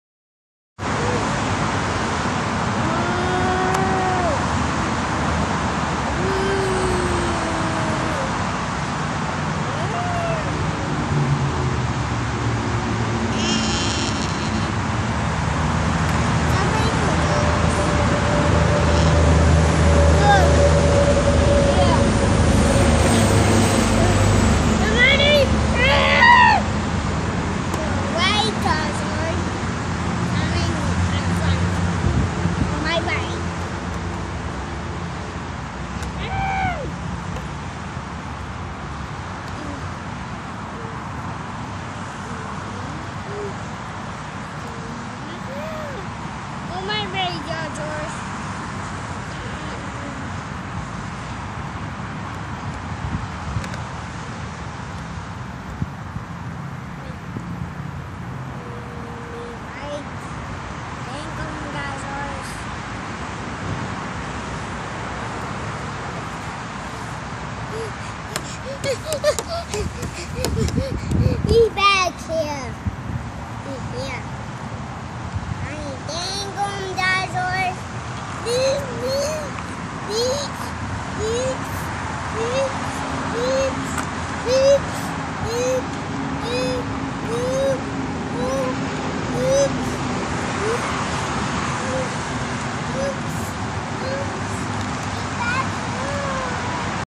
Daniel the dinosaur lover
Daniel is role playing with his dinosaurs